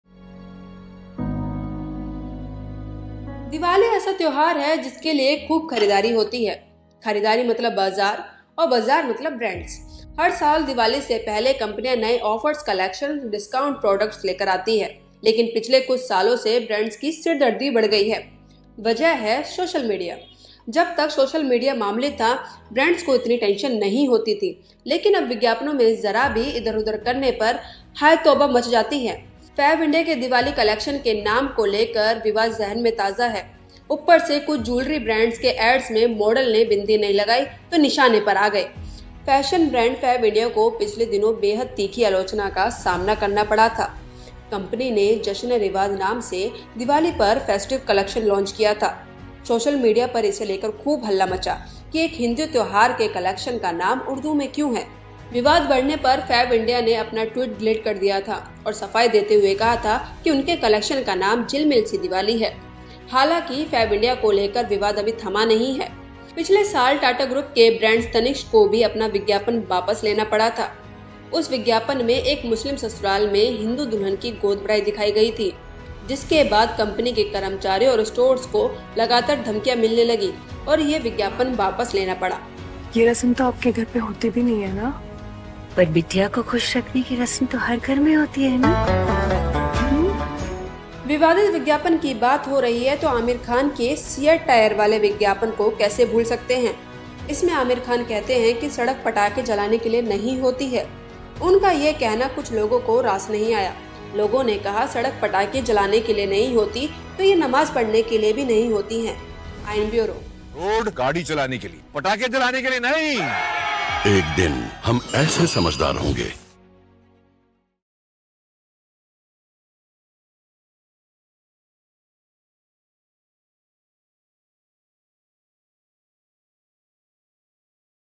Fabindia, Tanishq, CEAT... Diwali Ad Controversies
दिवाली के मौके पर कई कंपनियों ने लोगों को अपनी ओर खींचने के लिए ऐड्स जारी किए, लेकिन इन्हीं ऐड्स ने उन्हें मुसीबतों में डाल दिया।  फैबइंडिया के दीपावली कलेक्‍शन के नाम को लेकर विवाद जेहन में ताजा है... ऊपर से कुछ जूलरी ब्रैंड्स के ऐड्स में मॉडल्‍स ने बिंदी नहीं लगाई तो निशाने पर आ गए... देखें वीडियो